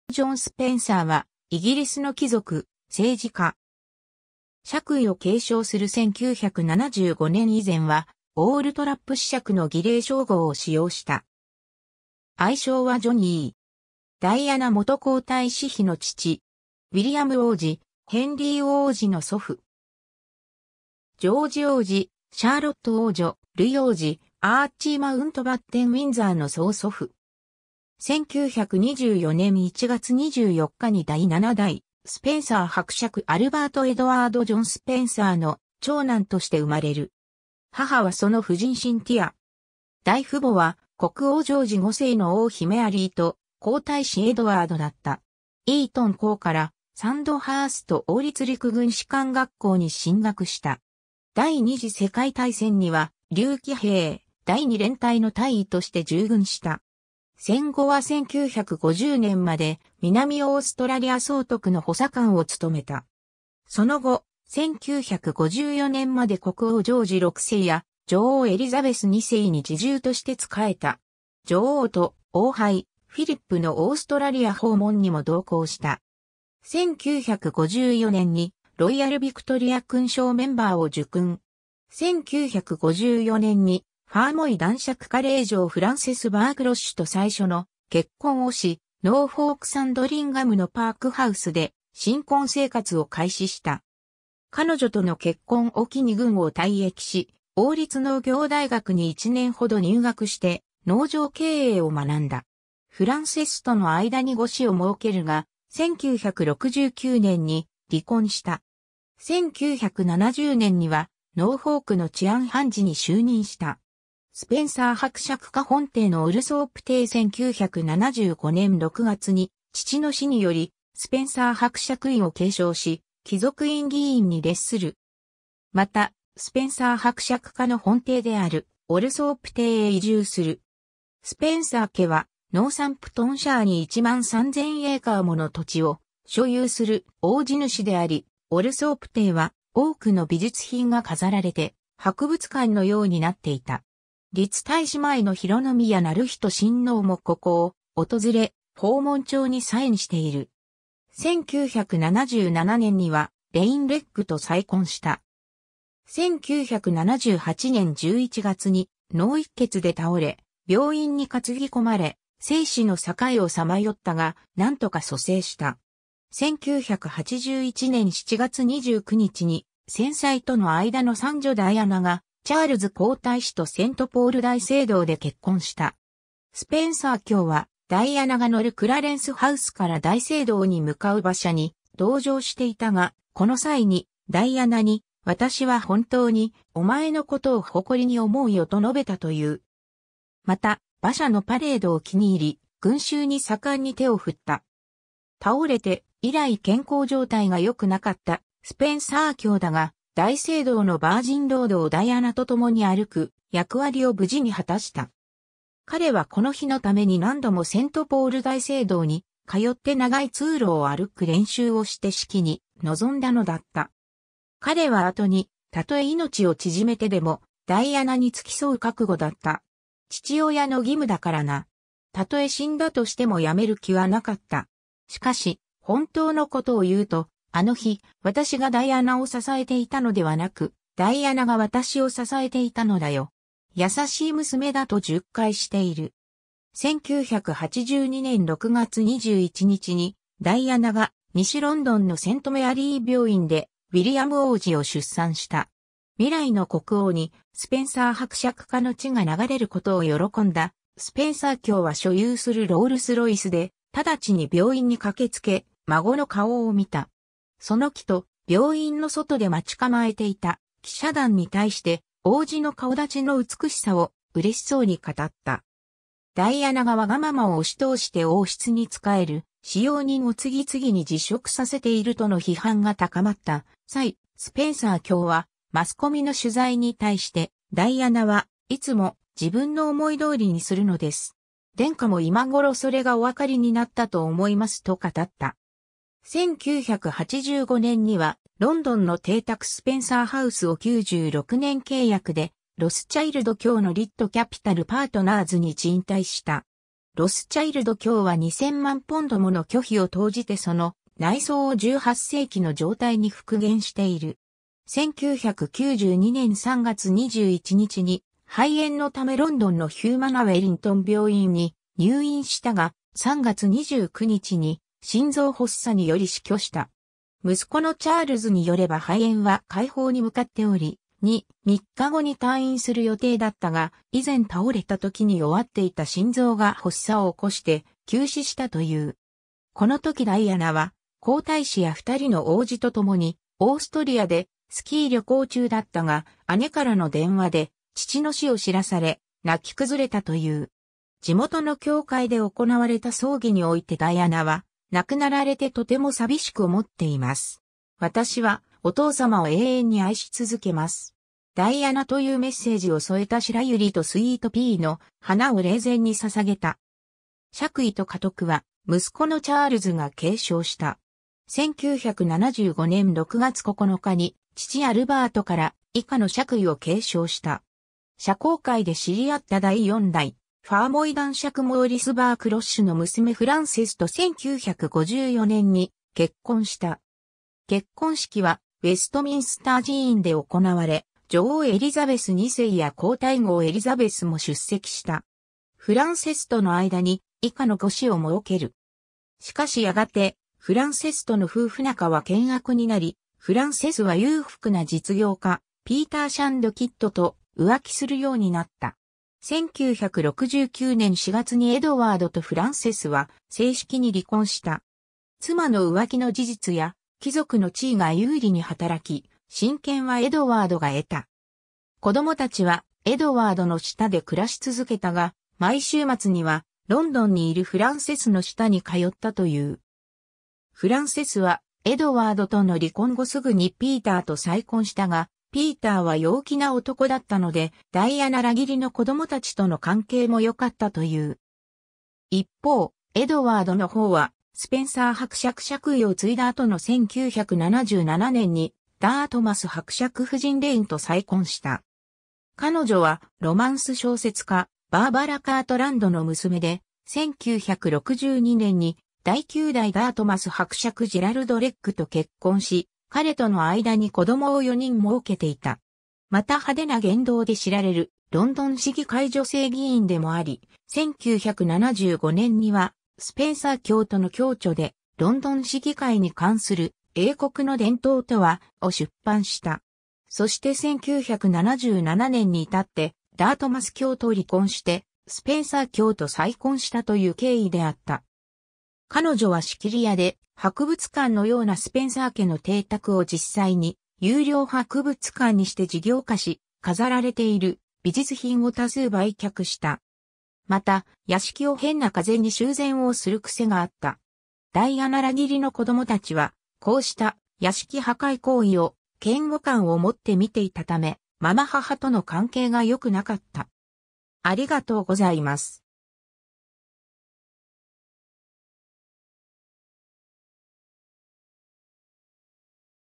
エドワード・スペンサー (第8代スペンサー伯爵)
スペンサー＝チャーチル家
第8代スペンサー伯爵エドワード・ジョン・スペンサー（英: Edward John Spencer, 8th Earl Spencer, MVO、1924年1月24日 - 1992年3月29日）は、イギリスの貴族、政治家。
 爵位を継承する1975年以前はオールトラップ子爵（Viscount Althorp）の儀礼称号を使用した。
愛称はジョニー（Johnnie）。
 ダイアナ元皇太子妃の父。
ウィリアム王子、ヘンリー王子の祖父。
ジョージ王子、シャーロット王女、ルイ王子、アーチー・マウントバッテン＝ウィンザーの曾祖父。
 1924年1月24日に第7代スペンサー伯爵アルバート・エドワード・ジョン・スペンサーの長男として生まれる。
母はその夫人シンティア（第3代アバコーン公爵ジェームス・ハミルトンの娘）。
代父母は国王ジョージ5世の王妃メアリーと皇太子エドワード（後の国王エドワード8世）だった。
 イートン校からサンドハースト王立陸軍士官学校に進学した。
第二次世界大戦には竜騎兵第2連隊の大尉として従軍した。
戦後は1950年まで南オーストラリア総督の補佐官を務めた。
 その後、1954年まで国王ジョージ6世や女王エリザベス2世に侍従として仕えた。
女王と王配フィリップのオーストラリア訪問にも同行した。
1954年にロイヤル・ヴィクトリア勲章メンバー（MVO）を受勲。
 1954年にファーモイ男爵家令嬢フランセス・バーク・ロッシュと最初の結婚をし、ノーフォーク・サンドリンガムのパーク・ハウスで新婚生活を開始した。
彼女との結婚を機に軍を退役し、王立農業大学に1年ほど入学して農場経営を学んだ。
フランセスとの間に5子を儲けるが、1969年に離婚した。
 1970年にはノーフォークの治安判事に就任した。
 スペンサー伯爵家本邸のオルソープ邸 1975年6月に父の死によりスペンサー伯爵位を継承し、貴族院議員に列する。
またスペンサー伯爵家の本邸であるオルソープ邸へ移住する。
スペンサー家はノーサンプトンシャーに1万3000エーカーもの土地を所有する大地主であり、オルソープ邸は多くの美術品が飾られて博物館のようになっていた。
立太子前の浩宮徳仁親王もここを訪れ、訪問帳にサインしている。
 1977年にはレイン・レッグと再婚した。
 1978年11月に脳溢血で倒れ、病院に担ぎ込まれ、生死の境をさまよったが、なんとか蘇生した。
 1981年7月29日に先妻との間の三女ダイアナがチャールズ皇太子とセントポール大聖堂で結婚した。
スペンサー卿は、ダイアナが乗るクラレンス・ハウスから大聖堂に向かう馬車に同乗していたが、この際にダイアナに「私は本当にお前のことを誇りに思うよ」と述べたという。
また馬車のパレードを気に入り、群衆に盛んに手を振った。
倒れて以来健康状態がよくなかったスペンサー卿だが、大聖堂のバージンロードをダイアナとともに歩く役割を無事に果たした。
彼はこの日のために何度もセントポール大聖堂に通って長い通路を歩く練習をして式に臨んだのだった。
彼は後に「たとえ命を縮めてでも、ダイアナに付き添う覚悟だった。父親の義務だからな。例え死んだとしても辞める気はなかった。しかし本当のことを言うと、あの日、私がダイアナを支えていたのではなく、ダイアナが私を支えていたのだよ。優しい娘だ」と述懐している。
 1982年6月21日にダイアナが西ロンドンのセント・メアリー病院でウィリアム王子を出産した。
未来の国王にスペンサー伯爵家の血が流れることを喜んだスペンサー卿は所有するロールス・ロイスでただちに病院に駆けつけ、孫の顔を見た。
その帰途、病院の外で待ち構えていた記者団に対して王子の顔立ちの美しさを嬉しそうに語った。
 ダイアナが我がままを押し通して王室に仕える使用人を次々に辞職させているとの批判が高まった際、スペンサー卿はマスコミの取材に対して「ダイアナはいつも自分の思い通りにするのです。殿下も今頃それがお分かりになったと思います」と語った。
 1985年にはロンドンの邸宅スペンサー・ハウスを96年契約でロスチャイルド卿のRIT・キャピタル・パートナーズに賃貸した。
ロスチャイルド卿は2000万ポンドもの巨費を投じてその内装を18世紀の状態に復元している。
 1...